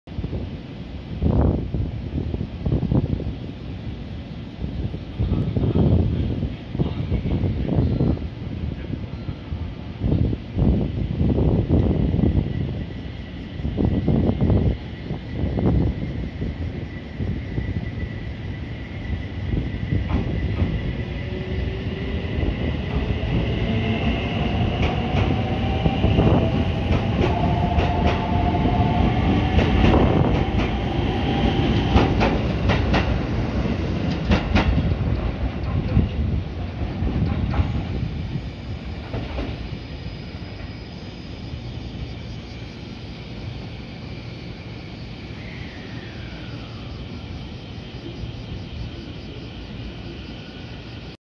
●20220718 313系 発車 ＠豊橋
撮影路線：JR東海 東海道本線
撮影駅：豊橋
行先：普通 浜松
車両：313系
両数：4両
撮影機材：【スマホ】SONY Xperia XZ2 Premium
MH：ーー
備考欄：